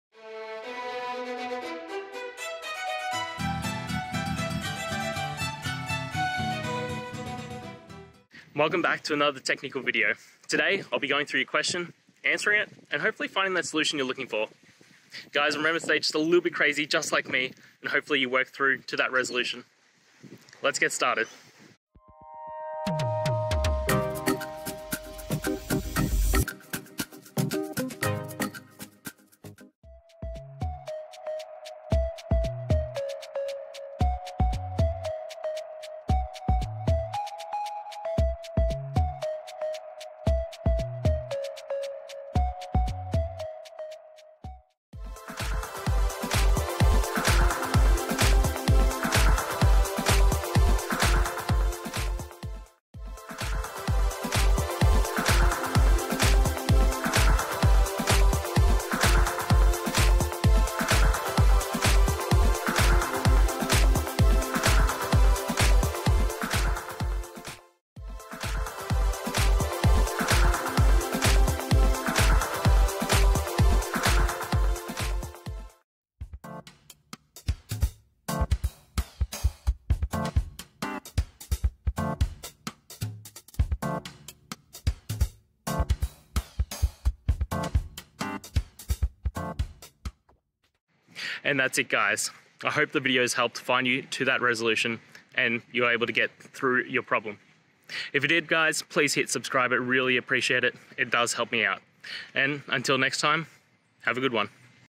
How to get client secret expiry date using the azure AD graph API
azure: How to get client secret expiry date using the azure AD graph API

Related to: azure, azure-active-directory, azure-ad-graph-api